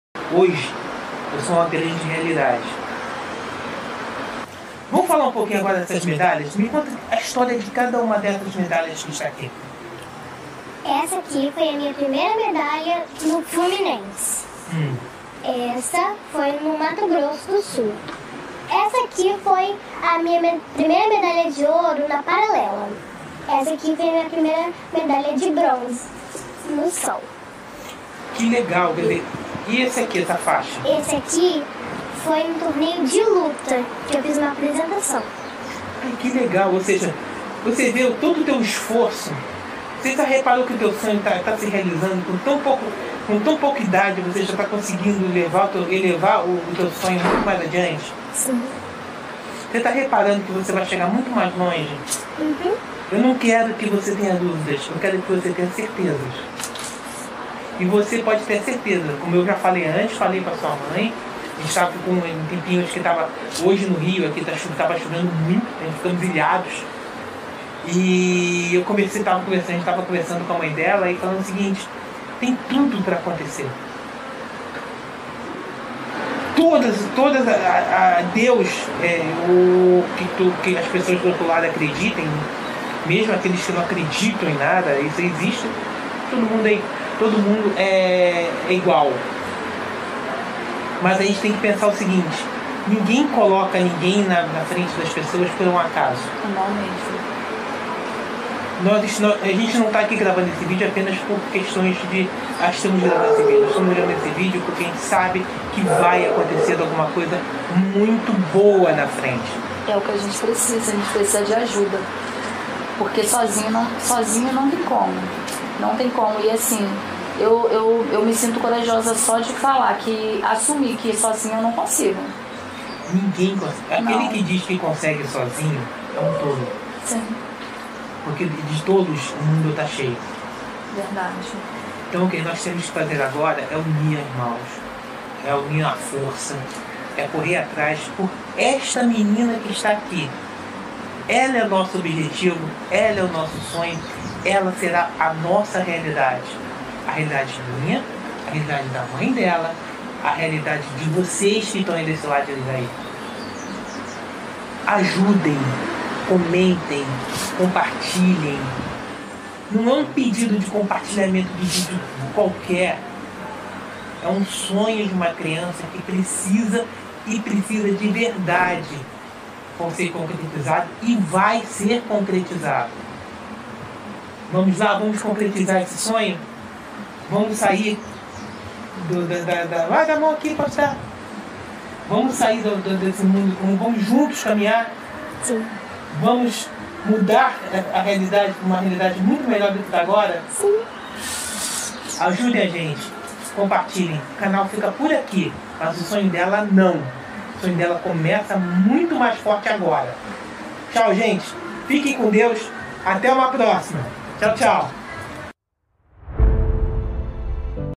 Maré: Como o apoio da comunidade pode transformar o mundo de uma criança
Na Comunidade da Maré, uma menina com grandes sonhos olímpicos está transformando sua realidade. Com o apoio e a força de sua comunidade, ela desafia as adversidades e segue determinada rumo ao seu objetivo. Este vídeo mostra como o amor, o incentivo e a união podem mudar o destino de uma criança, inspirando-a a alcançar alturas que antes pareciam impossíveis. Não perca essa história emocionante de superação e esperança!